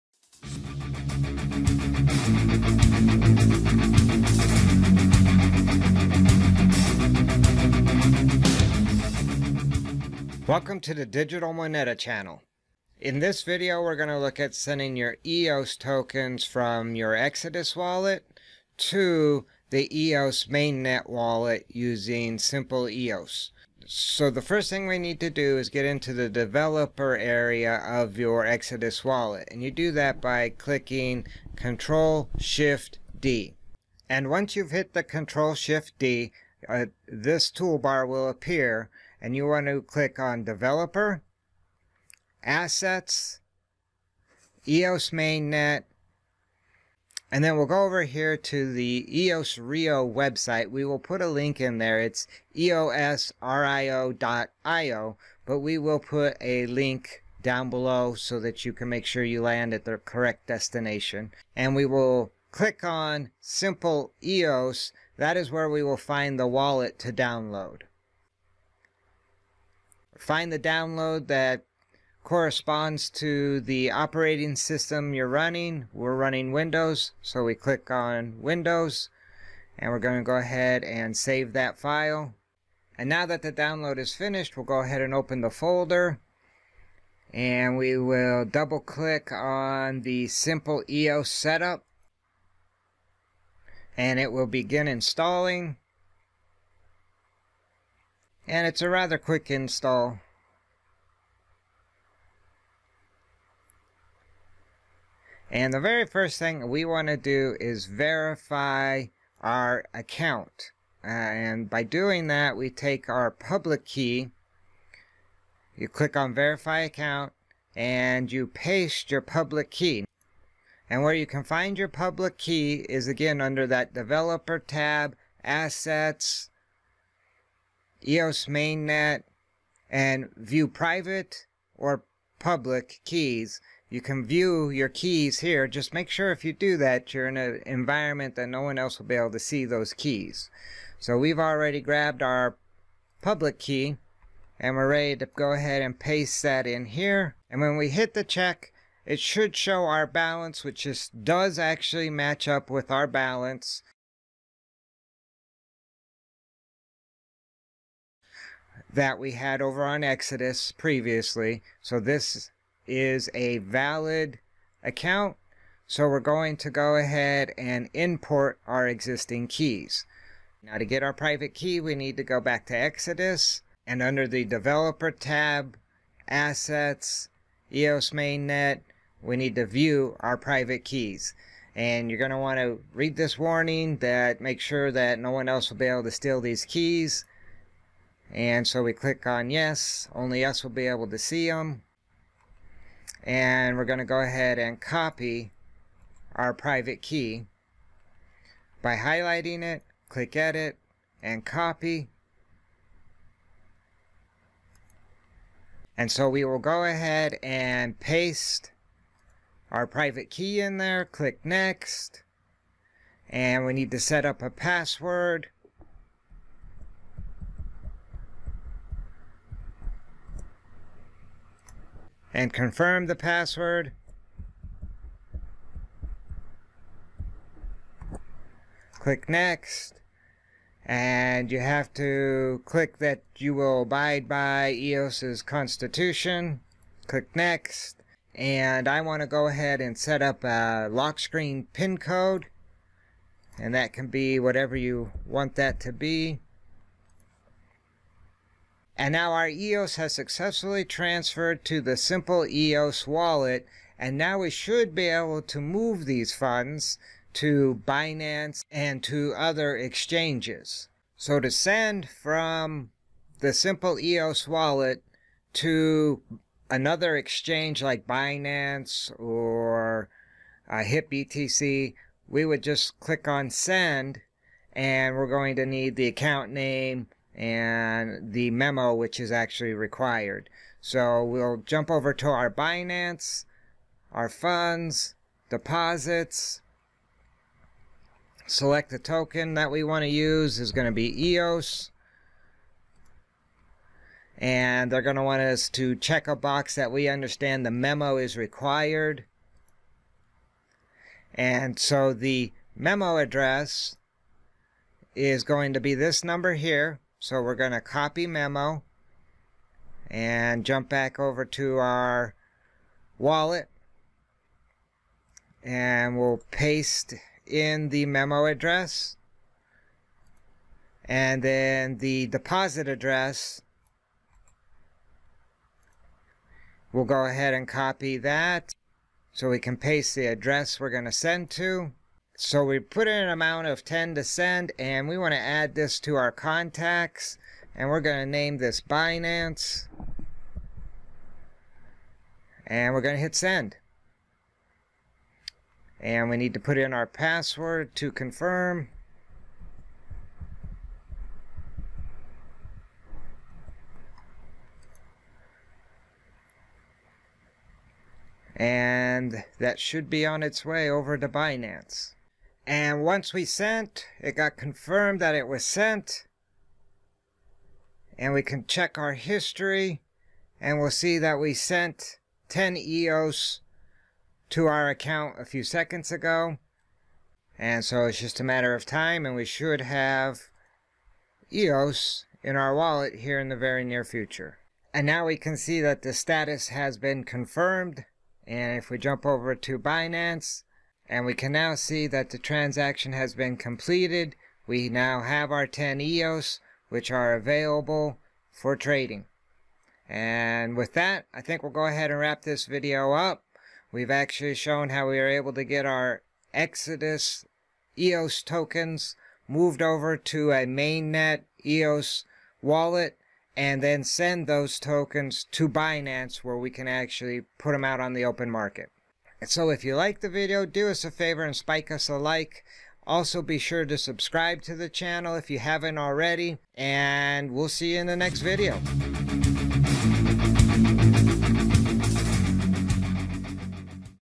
How to Transfer EOS Tokens from EXODUS to SimplEOS Mainet Wallet
How to Transfer EOS Tokens from EXODUS to SimplEOS Mainet Wallet. In addition, we will show you how to send EOS from the SimplEOS to your Binance exchange account.